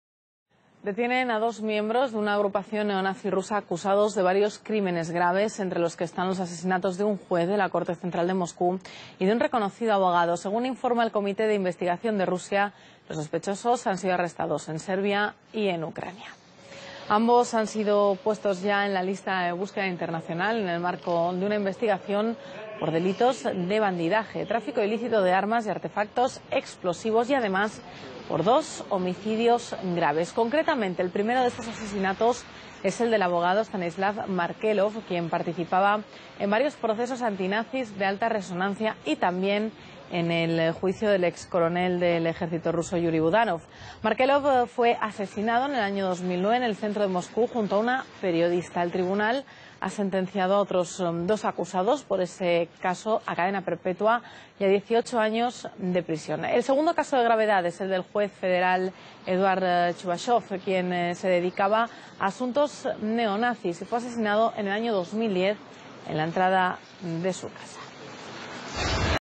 Detenido neonazi Ucraniano por el asesinato de un Juez Ruso
Han detenido en Ucrania a un miembro de una agrupación neonazi rusa, Mijaíl Vólkov. Está acusado de varios crímenes entre los cuales figura colaboración en el asesinato de un juez de la corte central de Moscú, Eduard Chuvashov.